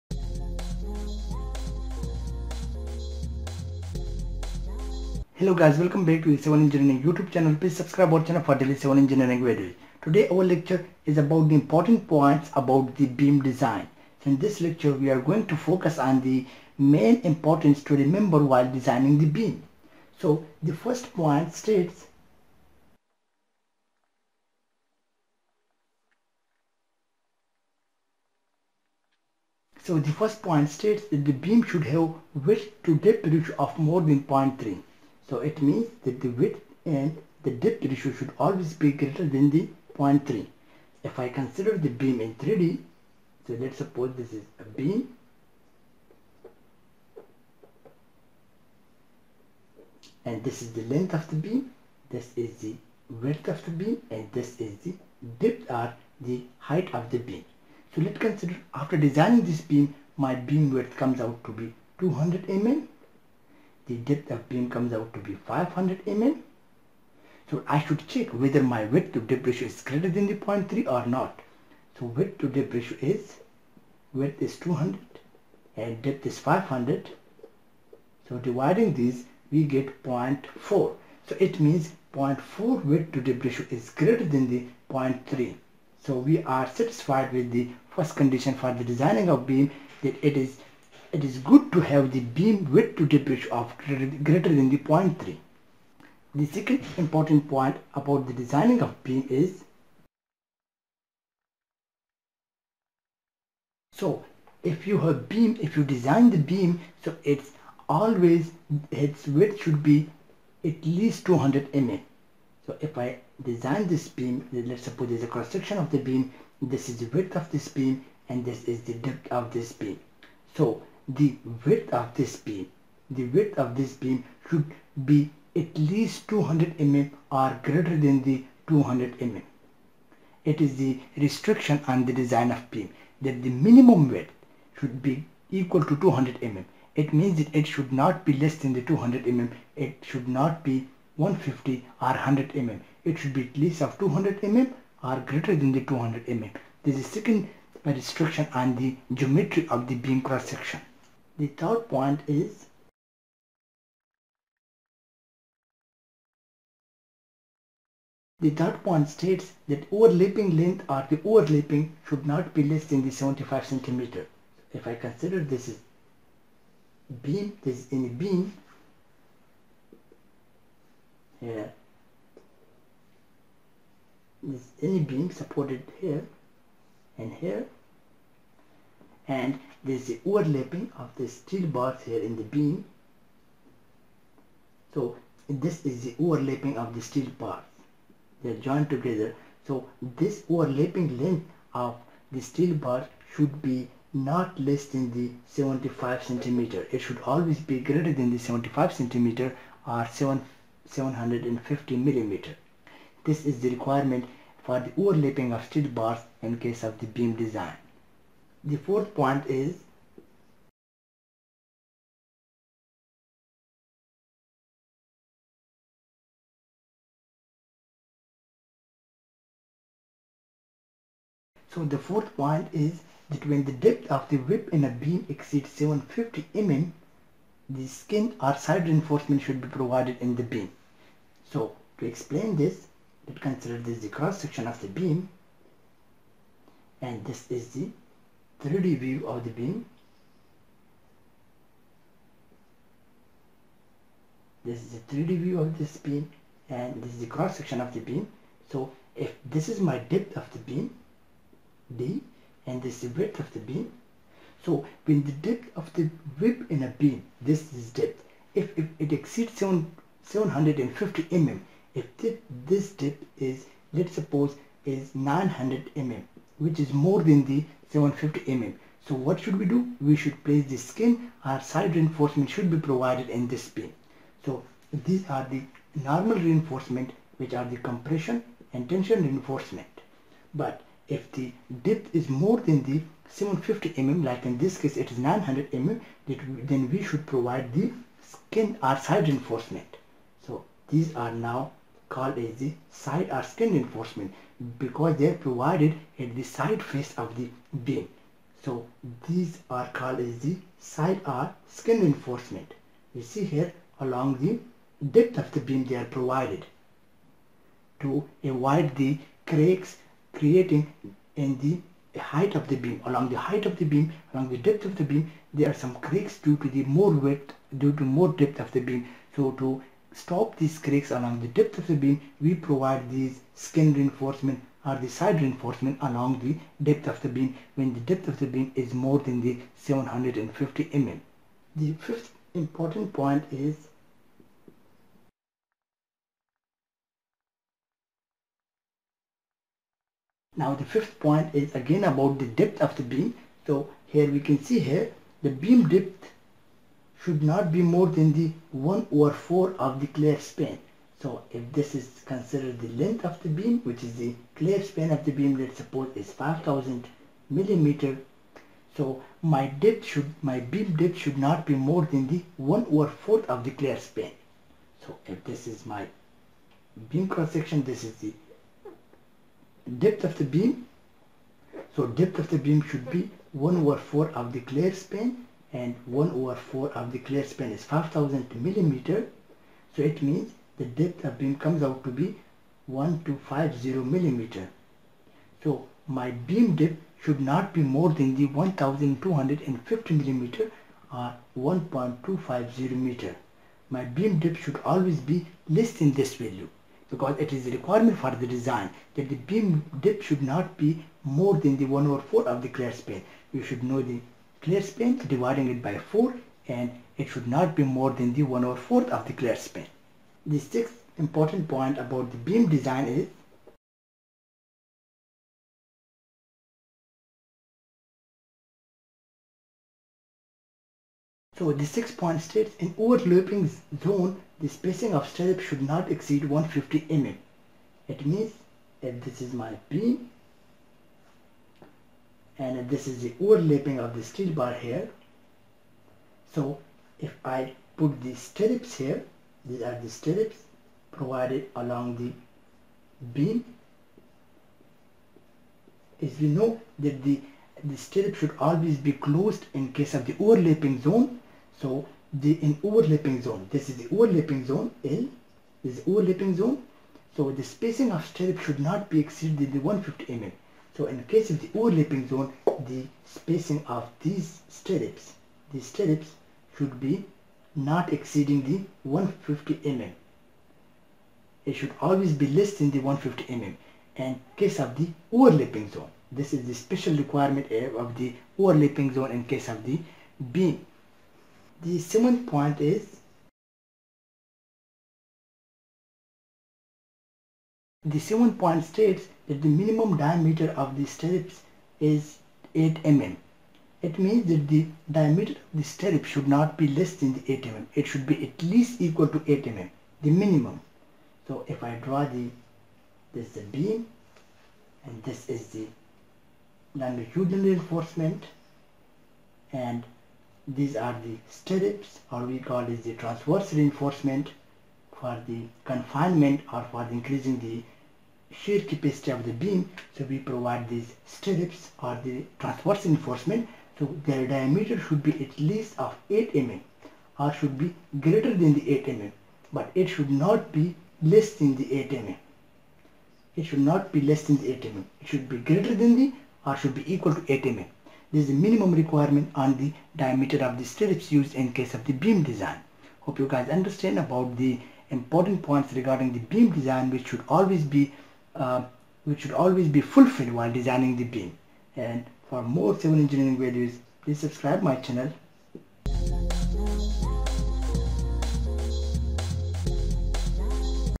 Important point about beam design
This video is about important points for beam design.

1. Beam should have width to depth ratio of more than 0.3.
2. Minimum width of beam = 20mm
3. Overlapping should not be less than75cm.
4. Beam depth should not be more than 1/4 of clear span.
5. In overlapping zone, spacing of stirrups should not exceed 150mm.